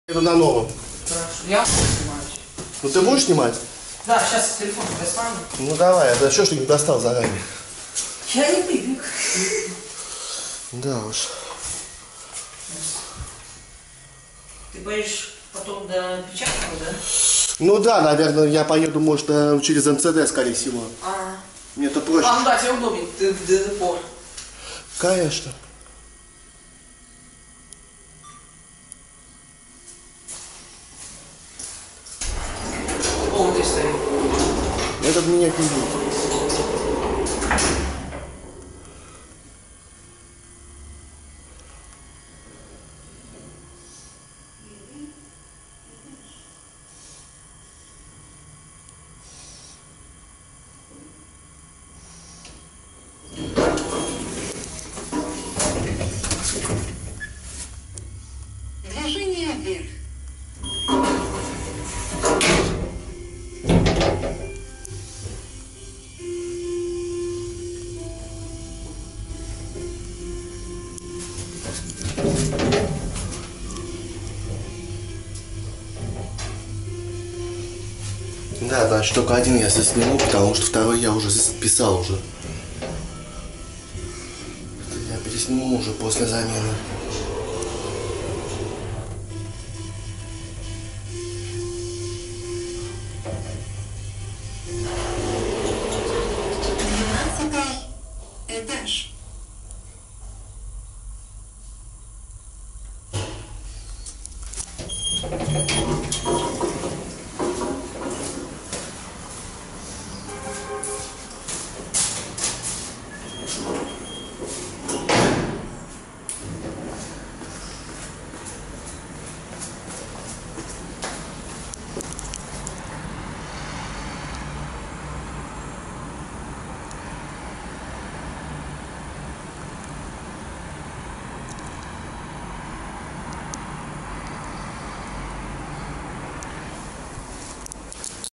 Пассажирский лифт V=1 м-с, Q=400 кг (3957), музыкальный лифт КМЗ
Кавказский бульвар 35/2к1
Второй лифт не заменён, снимал ранее, ссылка на этот лифт и до замены этого музыкального: